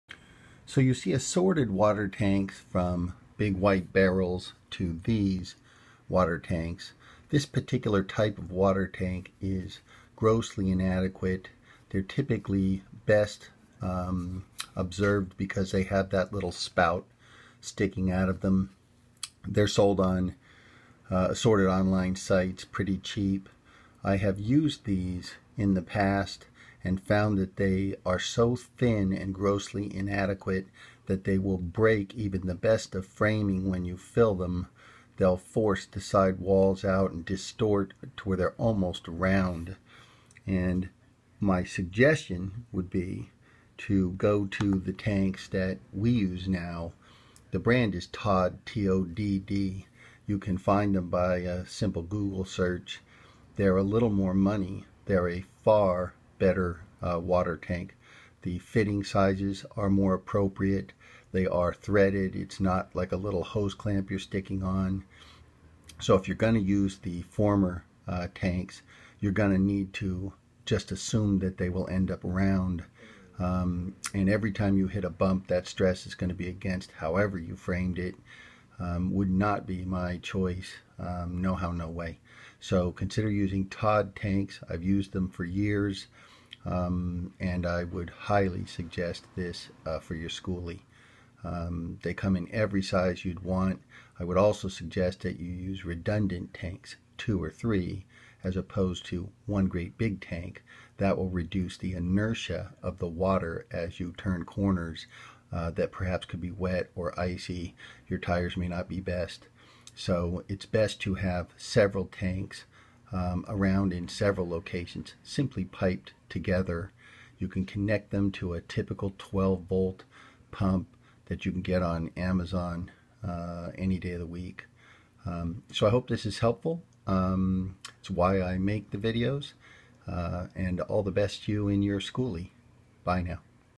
BAD SCHOOL BUS WATER TANKS !!!!!!!!!
School Bus Conversion Water Tanks. Please be careful in selecting your tank. Two topics here. One, the inertia of water as you turn corners if using large barrel types, and second, the horrible school bus water tanks sold on the online sites. The best water tanks for school bus conversions are Todd tanks. These will not distort and become round as they are filled, and will not bust your framing in your school bus tiny home. I had steel walls brake from these inferior tanks, and never used them again. I like to have several tanks, not one large tank. Installing water tanks in school bus conversions is easy and doesn't take up much space in square tanks, which hold more than round. The school bus water tanks from Todd have excellent fittings, and in the appropriate size threads for off the shelf fittings. I've frozen these (actually clients have) , along with some plumbing. Frozen solid. Once thawed, the pipes of coarse were no good, but the todd tanks survived with no issue.